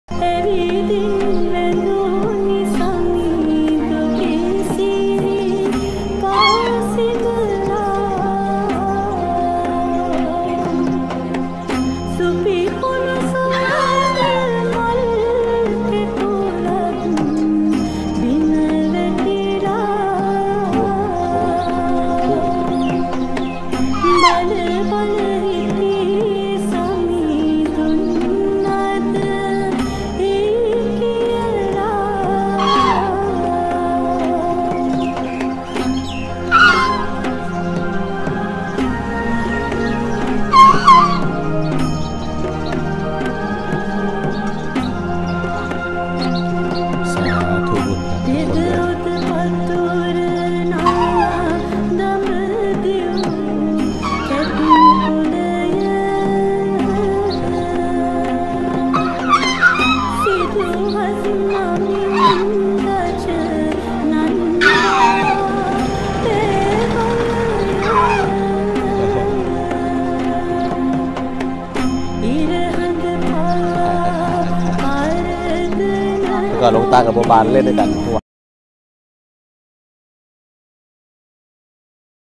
බලන්න මේ ඇත් පැටියාගේ ශ්‍රද්ධාව
මේ ඇත් පැටියෙක් ශ්‍රද්ධාව දැක්කම ඇස් දෙකට කඳුලු එනවා.බලන්න මොනතරම් හුරතල් ද?පින්වත් ඇත් පැටියෙක්.ඊළඟ ආත්මෙක මනුස්සයෙක් වෙලා ඉපදෙන්න කියලා ප්‍රාර්ථනා කරගෙන යමු.